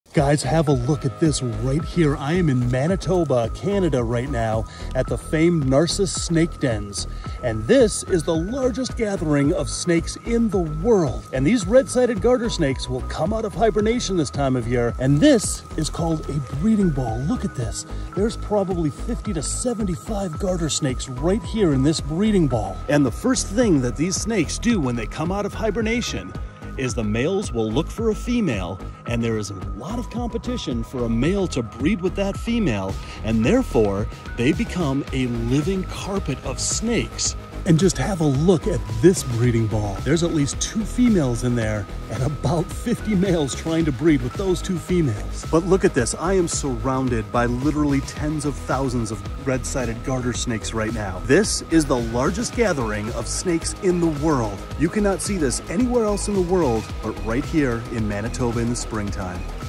Largest Gathering of Snakes in the World!
At the famous Narcisse Snake Dens in Manitoba is the largest gather of snakes in the world where tens of thousands of garter snakes emerge in the spring and begin breeding!